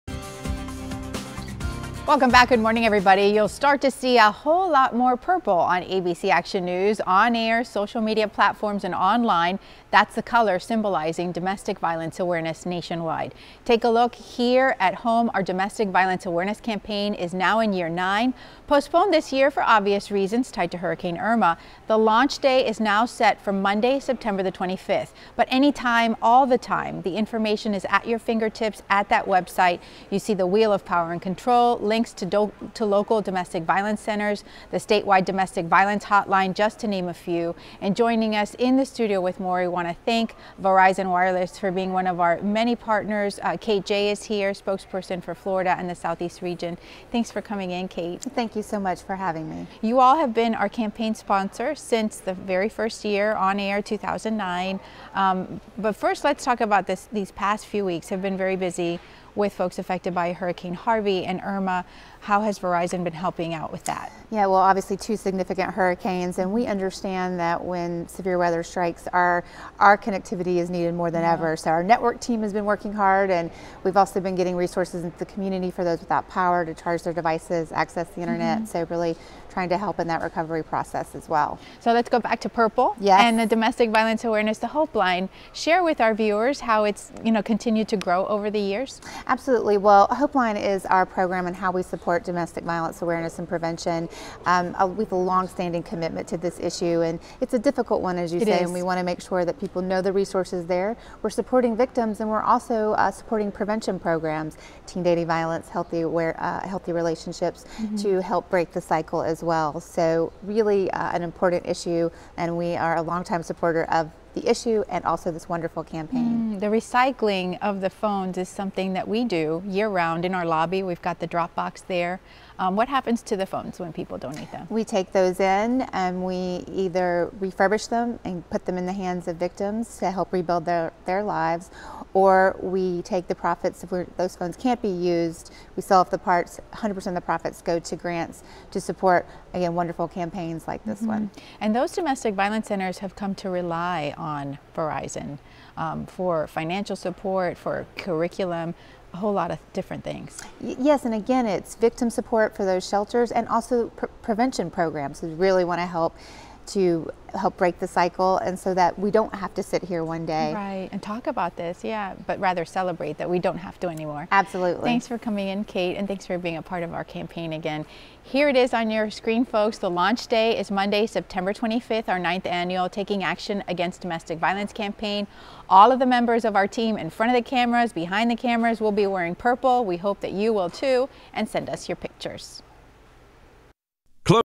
Positively Tampa Bay: Verizon
New launch date for 9th annual Taking Action Against Domestic Violence campaign! Verizon's Kate Jay explains the company's commitment to DV awareness and the ABC Action News campaign!